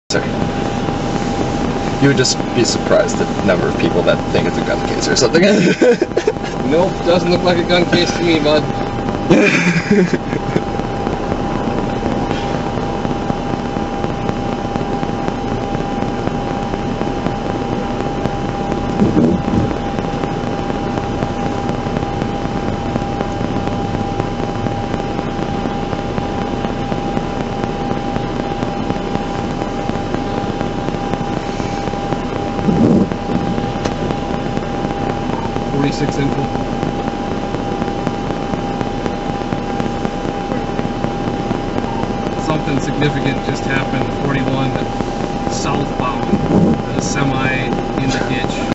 2019 1031 I-41
On October 31, 2019 for the 8 hour span between roughly 5:00am and 1:00pm we recorded 20 crashes and 13 cars in the ditch within the Sheriff's Office jurisdiction. While this was average amount for a snow day, and we had a lot of help dealing with these crashes, this was an atypical day in general.  Of course, the early snowfall date itself it noteworthy, and we don’t think people appropriately anticipated the poor road conditions. Additionally, of those 20 crashes, 5 of them involved semi-tractor trailer units. Thankfully, there were only a few people with minor injuries reported in handful of those crashes.

In particular, we have no idea how this crash, a portion of which is captured on squad video, did not result in any significant injuries or fatalities. Deputy Rippl was investigating a crash on I-41SB near Oakridge Rd when his squad camera captured another crash in the distance.  Watch closely-- and you may have to watch it a few times. A semi-tractor trailer was involved in an I-41 NB crash, which halted traffic and sent a passenger car into the east side ditch; and then quickly sent the semi-tractor trailer OVER AND THROUGH THE MEDIAN WALL, across all travel lanes of I-41 SB, and into the west side ditch. Thankfully, all of the vehicles traveling on I-41 SB had appropriately slowed for the squad investigating the first crash, and did not have the opportunity to get up to highway speed, which would have been completely inappropriate driving behavior for yesterday's weather.

**Side note - Deputy Rippl was talking to a driver (about his clarinet case) who was seated in the backseat of his squad from the first crash. He did not see the semi incident occur, but rather first noticed the absence of NB traffic, which prompted him to look further, and then quickly reported a “significant incident” involving a semi.

So our PSA for the day??  Winter is here. SLOW DOWN in inclement weather.